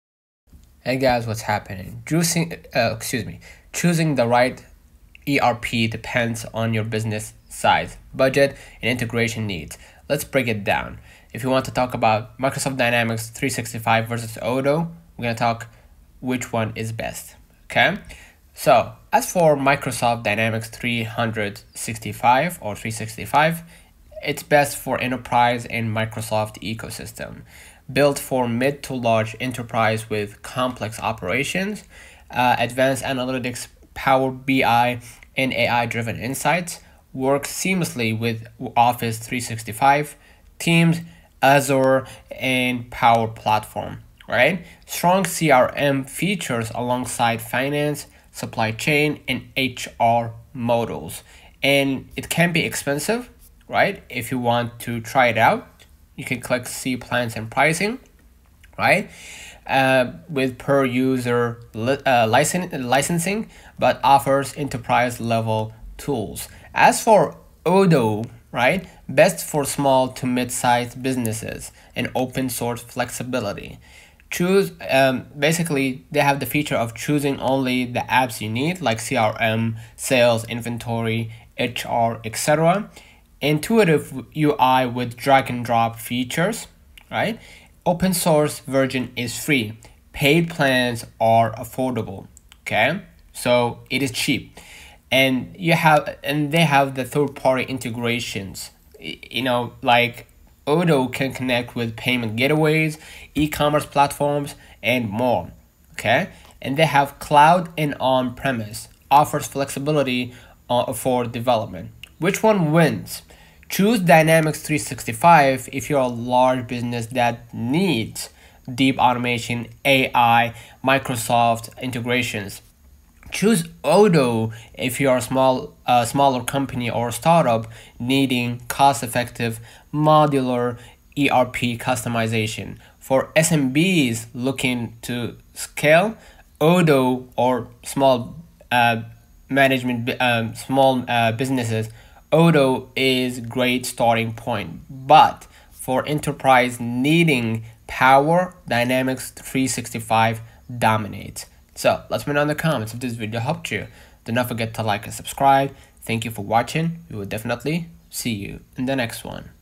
MICROSOFT DYNAMICS 365 VS ODOO | Best ERP Software Comparison
Microsoft Dynamics 365 vs Odoo? In this video, we break down Odoo vs Dynamics 365 to help you choose the right software for your business. Microsoft Dynamics 365 offers powerful enterprise solutions, while Odoo provides flexibility and affordability. If you're wondering about Odoo vs Microsoft Dynamics 365, we’ll cover their features, pricing, and ease of use. Watch this dynamics 365 review to see which one fits your needs best.